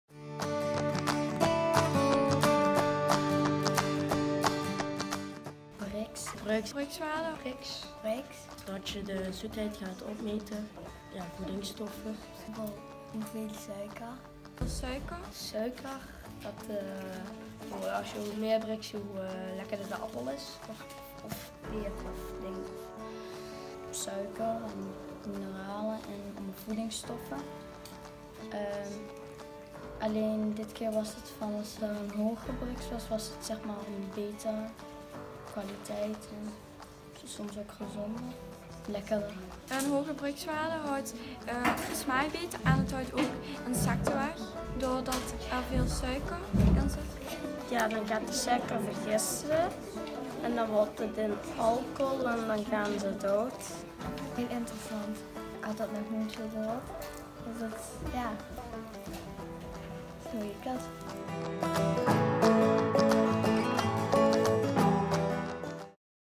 Brix Guest Lesson - Aptus Education
Aptus education found its way into the classroom! We had the chance to teach the cool kids of the Emile Wesly school in Maastricht what Brix is, why it's important, and how to measure it. After that, we checked how much stuck with them... Turns out they did quite good!🤓👍